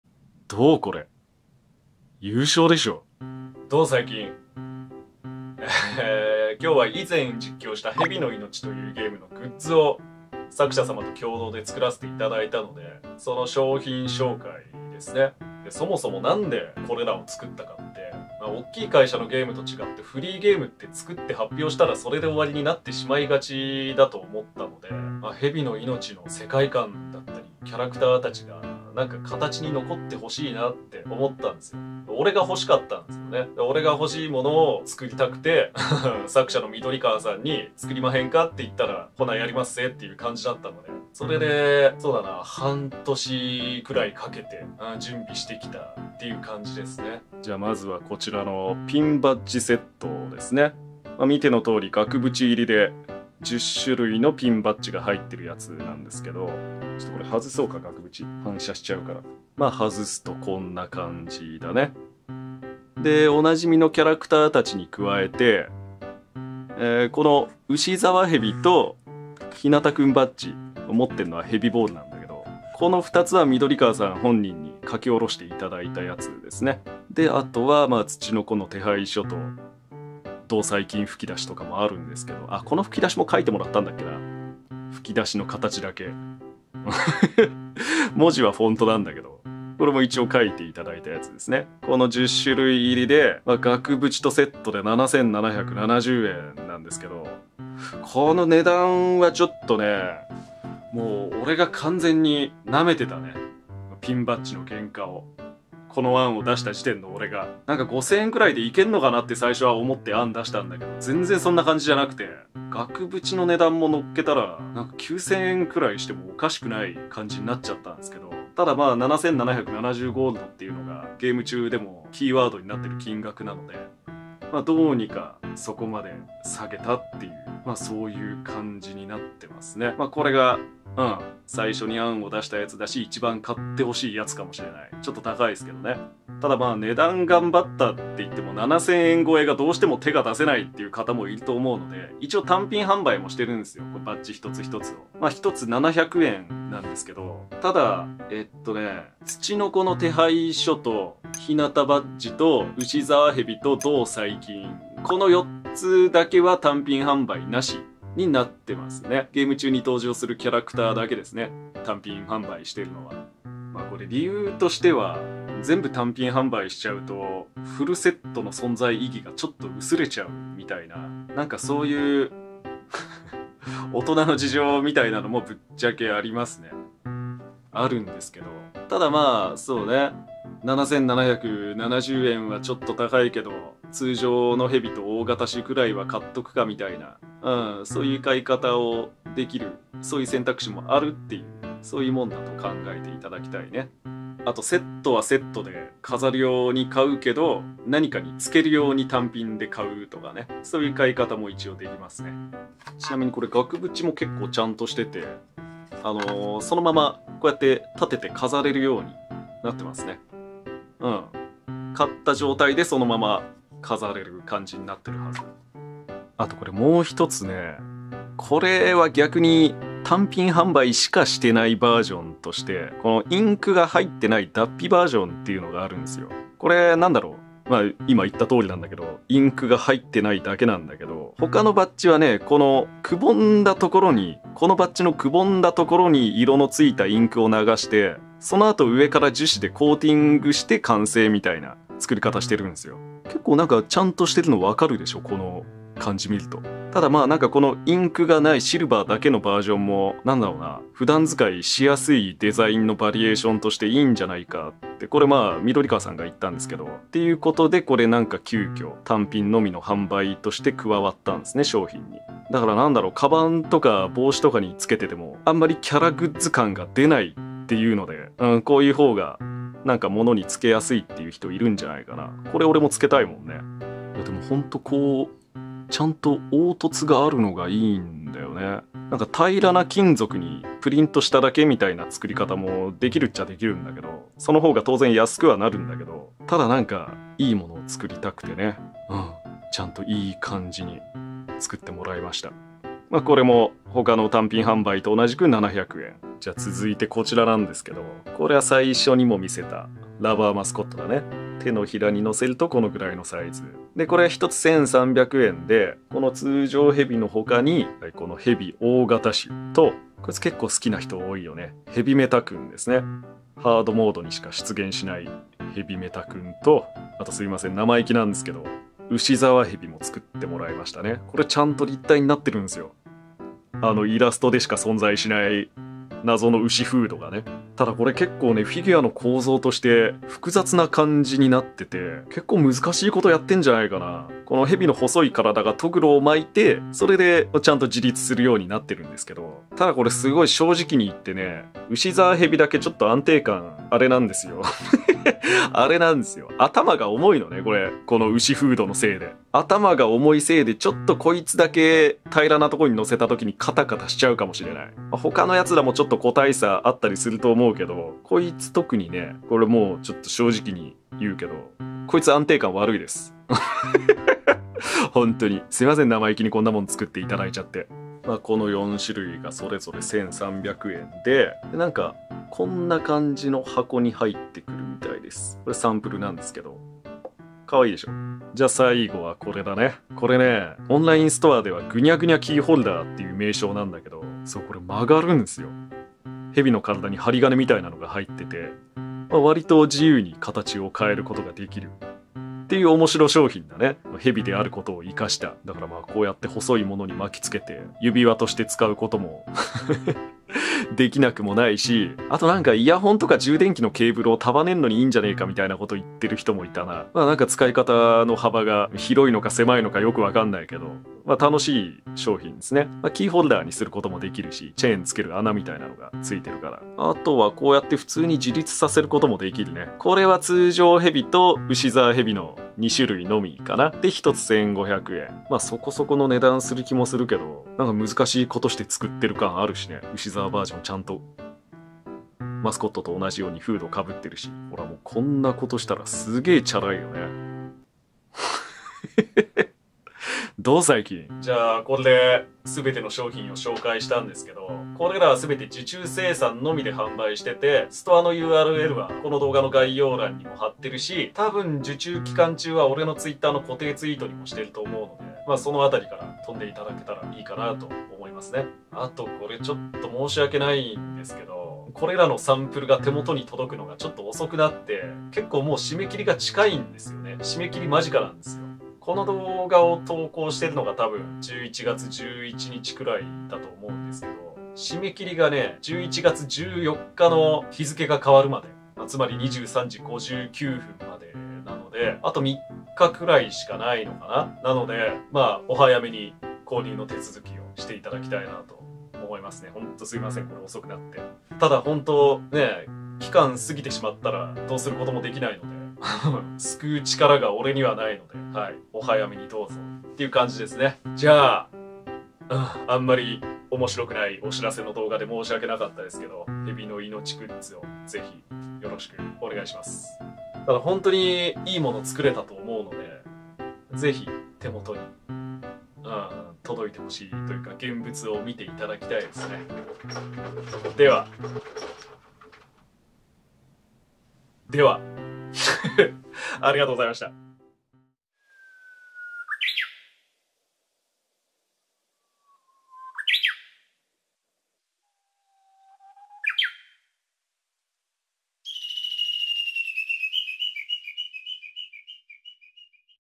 『ヘビの命』がグッズになりました。※受付終了しました
いい仕上がりだと思います。
※テロップではピンバッジセットが7700円と表記されていますが正しくは7770円です。